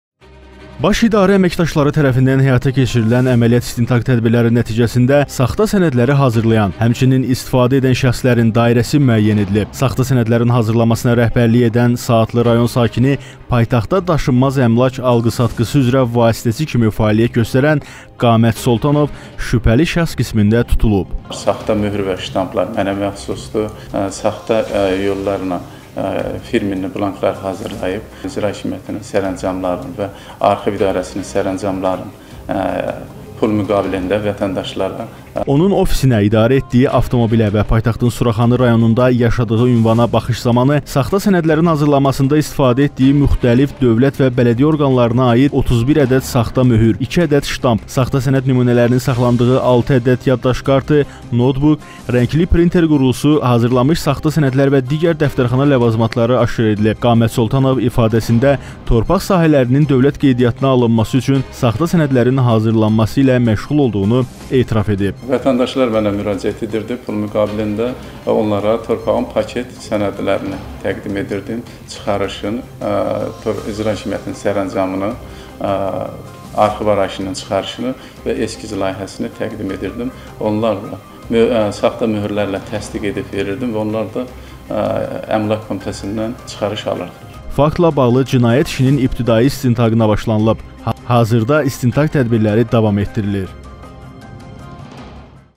SAXTA ƏMLAK SƏNƏDİ DÜZƏLDƏN ƏLƏ KEÇDİ
Ayrı-ayrı vətəndaşları torpaq sahələrini dövlət qeydiyyatına almaq adı ilə aldadıb